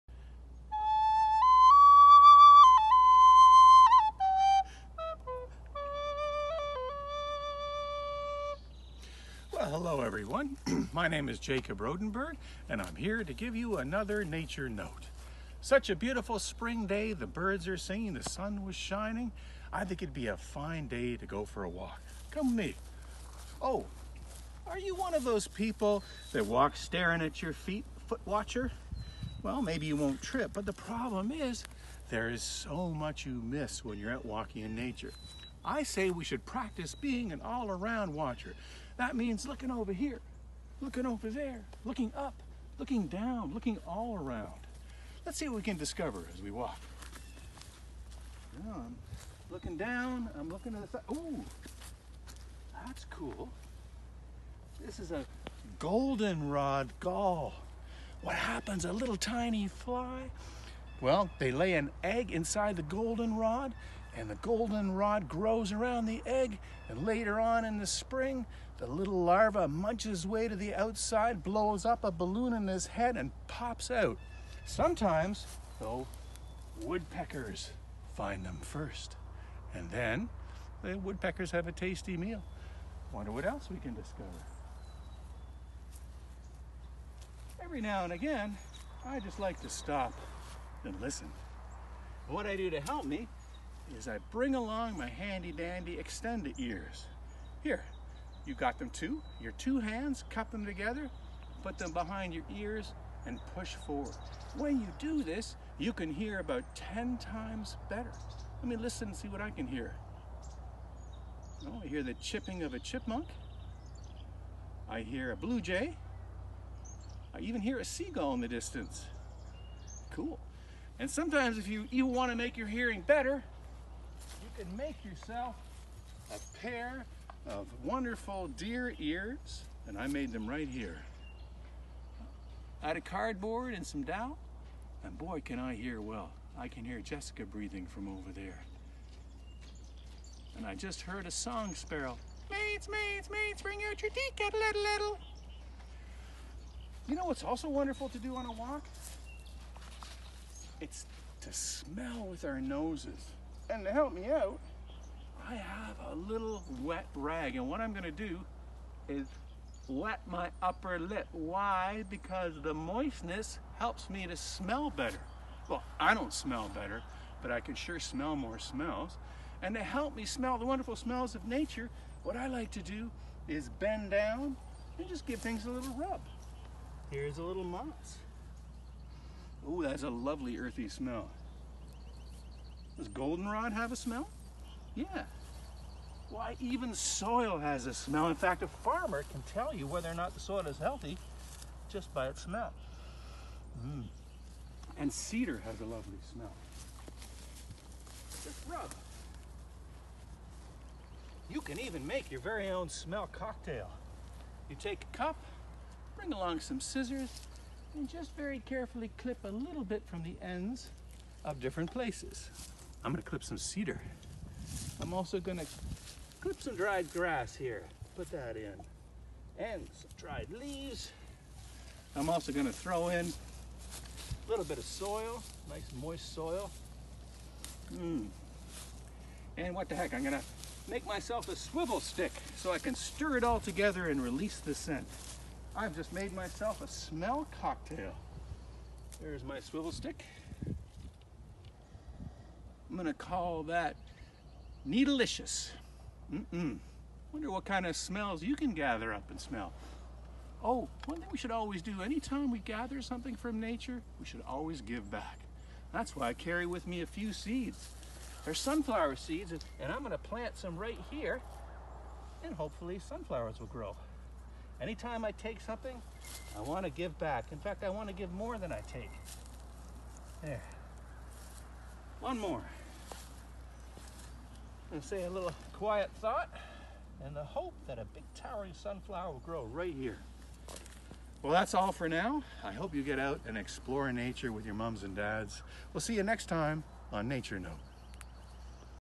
Nature Note 4: Using Your Senses
Join Executive Director, Jacob Rodenburg, for a lunch time Nature Note! Today he explores how you can use your sense to enjoy and explore nature. Learn some great tips and take yourself outside to try them!